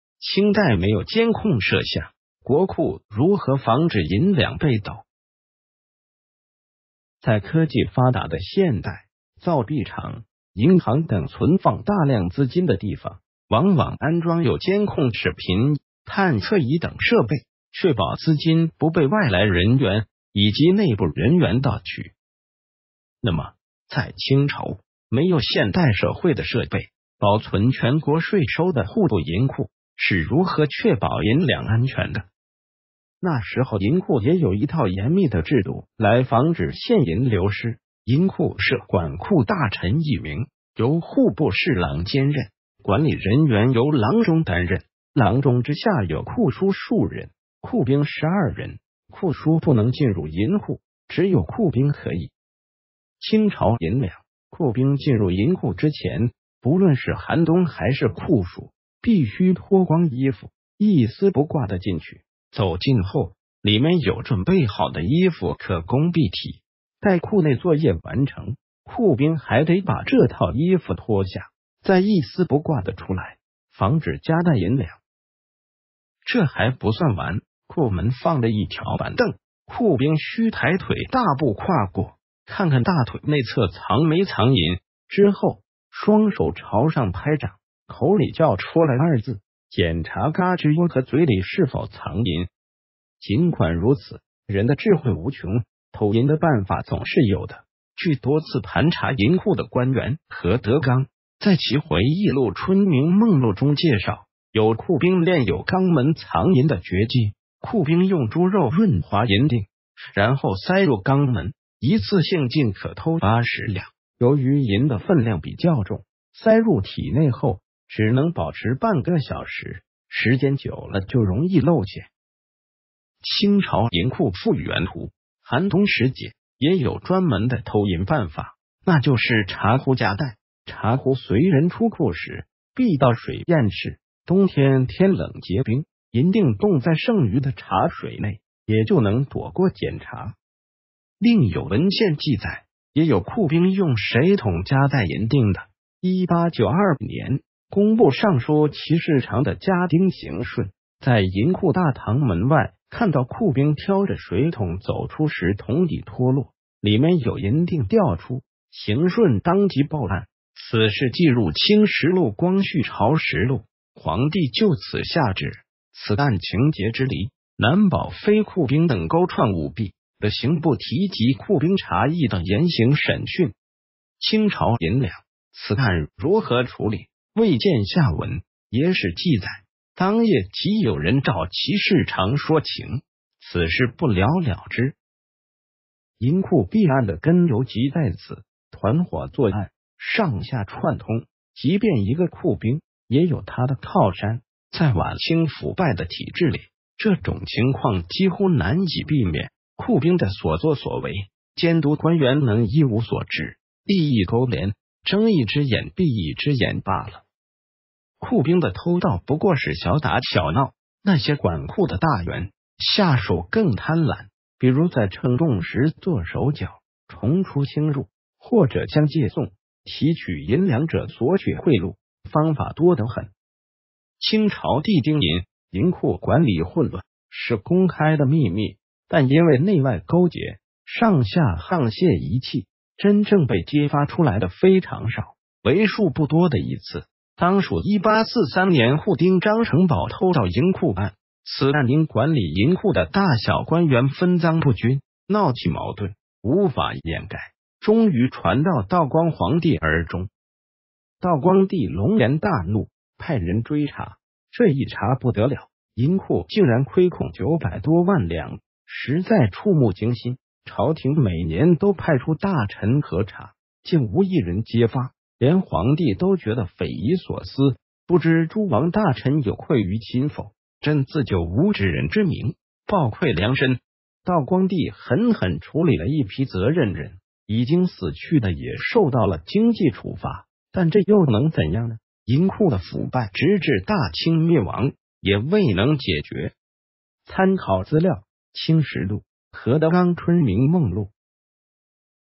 清代没有监控摄像, 国库如何防止银两被盗?
在科技发达的现代，造币厂、银行等存放大量资金的地方，往往安装有监控视频、探测仪等设备，确保资金不被外来人员以及内部人员盗取。
那么，在清朝，没有现代社会的设备，保存全国税收的户部银库，是如何确保银两安全的？
那时候银库也有一套严密的制度来防止现银流失。银库设管库大臣一名，由户部侍郎兼任，管理人员由郎中担任；郎中之下有库书数人，库兵12人。库书不能进入银库，只有库兵可以。

清朝银两。
库兵进入银库之前，不论是寒冬还是酷暑，必须脱光衣服，一丝不挂地进去。走进后，里面有准备好的衣服可供蔽体。待库内作业完成，库兵还得把这套衣服脱下，再一丝不挂地出来，防止夹带银两。
这还不算完，库门放着一条板凳，库兵需抬腿大步跨过，看看大腿内侧藏没藏银；之后，双手朝上拍掌，口里叫“出来”二字，检查胳肢窝和嘴里是否藏银。
尽管如此，人的智慧无穷，偷银的办法总是有的。据多次盘查银库的官员何德刚在其回忆录《春明梦录》中介绍，有库兵练有肛门藏银的绝技。库兵用猪肉润滑银锭，然后塞入肛门，一次性竟可偷80两！由于银的分量比较重，塞入体内后，只能保持半个小时，时间久了就容易露馅。

清朝银库复原图。
寒冬时节，也有专门的偷银办法，那就是茶壶夹带。茶壶随人出库时，必倒水验视，冬天天冷结冰，银锭冻在剩余的茶水内，也就能躲过检查。
另有文献记载，也有库兵用水桶夹带银锭的。1892年，工部尚书祁世长的家丁邢顺，在银库大堂门外看到库兵挑着水桶走出时桶底脱落，里面有银锭掉出，邢顺当即报案。此事记入《清实录·光绪朝实录》，皇帝就此下旨：“此案情节支离，难保非库兵等勾串舞弊，著刑部提集库兵茶役等严行审讯。”

清朝银两。
此案如何处理，未见下文，野史记载，当夜即有人找祁世长说情，此事不了了之。
银库弊案的根由即在此，团伙作案，上下串通，即便一个库兵，也有他的靠山。在晚清腐败的体制里，这种情况几乎难以避免。库兵的所作所为，监督官员能一无所知？利益勾连，睁一只眼闭一只眼罢了。
库兵的偷盗不过是小打小闹，那些管库的大员，下手更贪婪。比如在称重时做手脚，重出轻入，或者向解送、提取银两者索取贿赂，方法多得很。

清朝地丁银。
银库管理混乱，是公开的秘密，但因为内外勾结、上下沆瀣一气，真正被揭发出来的非常少。为数不多的一次，当属1843年库丁张诚保偷盗银库案。此案因管理银库的大小官员分赃不均，闹起矛盾，无法掩盖，终于传到道光皇帝耳中。
道光帝龙颜大怒，派人追查，这一查不得了，银库竟然亏空900多万两，实在触目惊心！朝廷每年都派出大臣核查，竟无一人揭发，连皇帝都觉得匪夷所思：“不知诸王大臣有愧于心否？朕自咎无知人之明，抱愧良深”。
道光帝狠狠处理了一批责任人，已经死去的也受到了经济处罚。但这又能怎样呢？银库的腐败直至大清灭亡，也未能解决。
参考资料：《清实录》，何德刚《春明梦录》